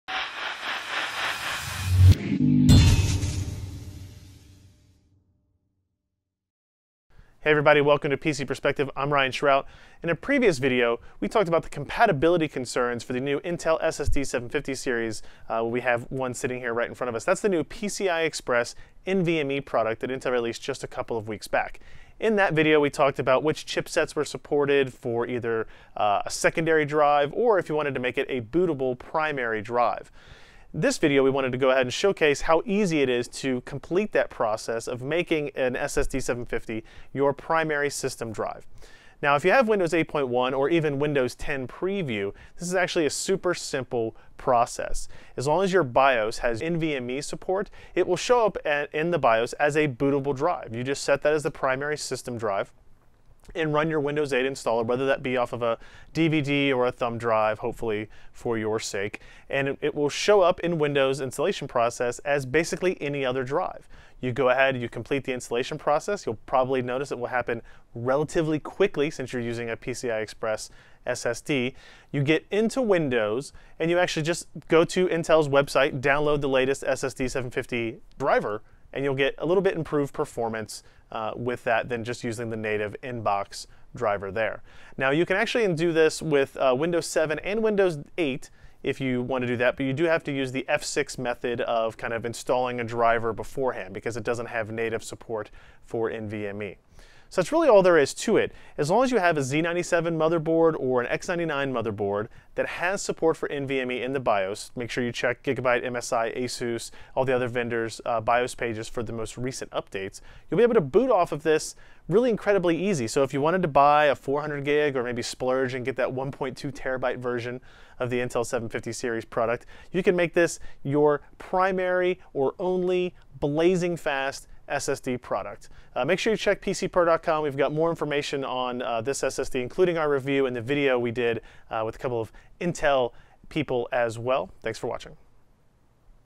Installing Windows to the Intel SSD 750 Series
In very early April, Intel put a shot across the bow of the storage world with the release of the SSD 750 Series of storage devices. Using the PCI Express bus but taking advantage of the new NVMe (Non-Volatile Memory Express) protocol, it drastically upgrades the capabilities of storage within modern PC platforms. In Allyn's review, for example, we saw read data transfer rates cross into the 2.6 GB/s range in sequential workloads and write rates over 1.2 GB/s sequentially. Even more impressive is the random I/O performance where the SSD 750 is literally 2x the speed of previous PCIe SSD options.

A couple of weeks later we posted a story looking into the compatibility of the SSD 750 with different motherboards and chipsets. We found that booting from the SSD 750 Series products is indeed going to require specific motherboards and platforms simply due to the "new-ness" of the NVMe protocol. Officially, Intel is only going to support Z97 and X99 chipsets today but obviously you can expect all future chipsets to have proper NVMe integration. We did find a couple of outliers that allowed for bootability with the SSD 750, but I wouldn't count on it.

Assuming you have a Z97/X99 motherboard that properly supports NVMe drives, of which ASUS, MSI and Gigabyte seem to be on top of, what are the steps and processes necessary to get your system up and running on the Intel SSD 750? As it turns out, it's incredibly simple.